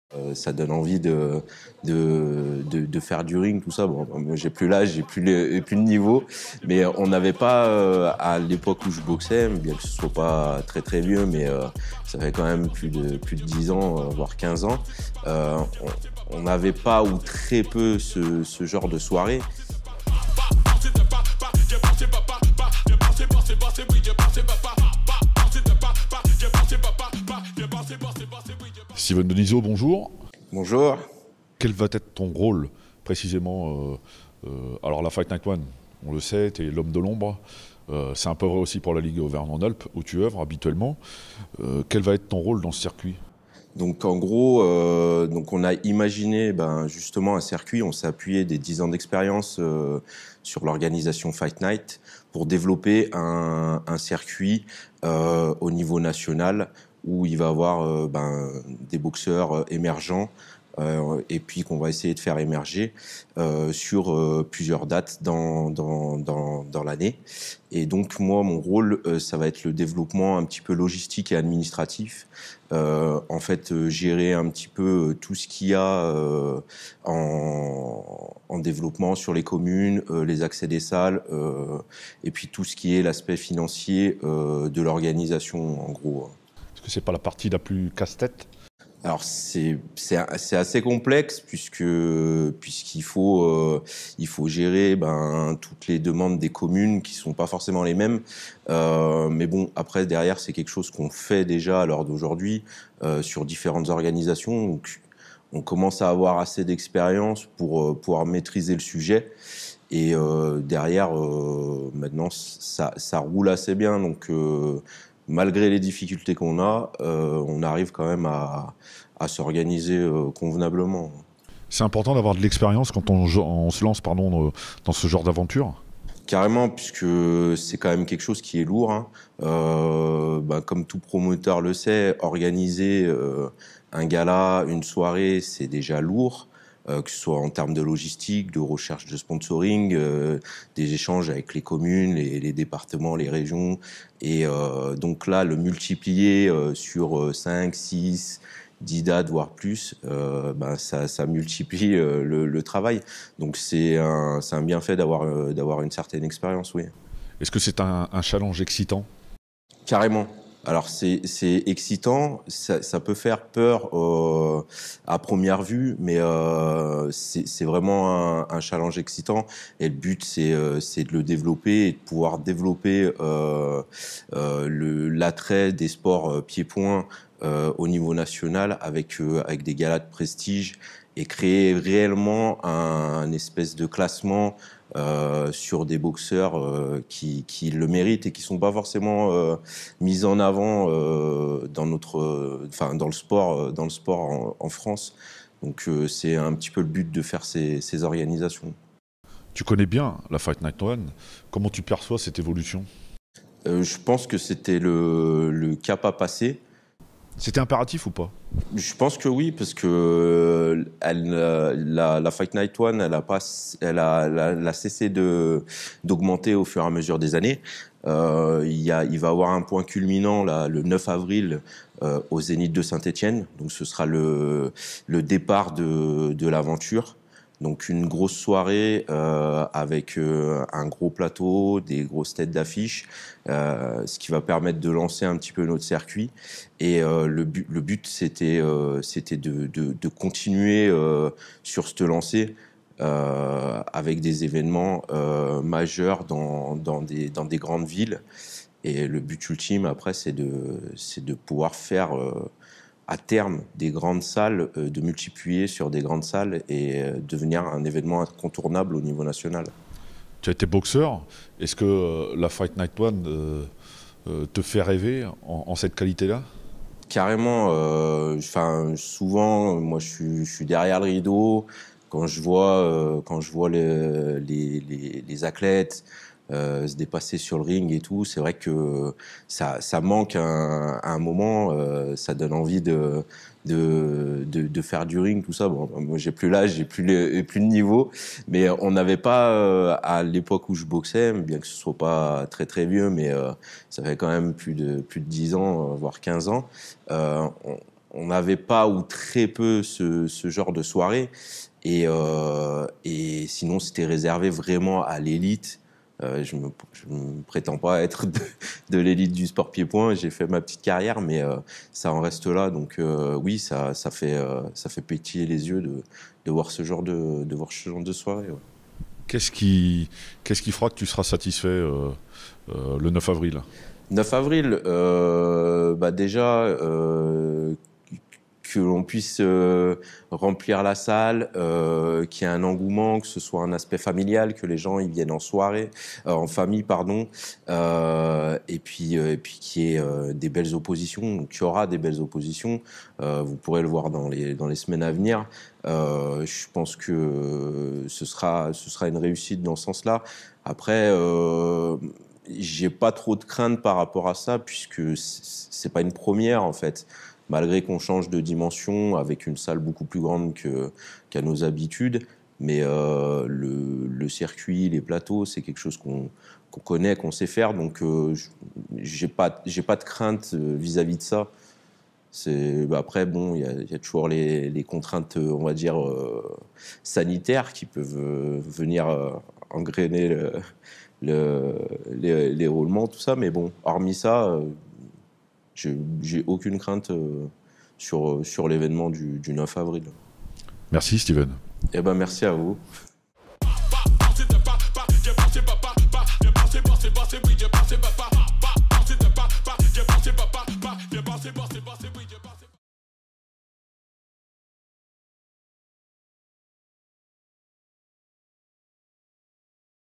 Fight Night One : itw Steven Denizot
Steven Denizot est l'homme de l'ombre mais la véritable tour de contrôle de la Fight Night One Organisation. En charge des finances et de la logistique, il explique ce que sera son rôle.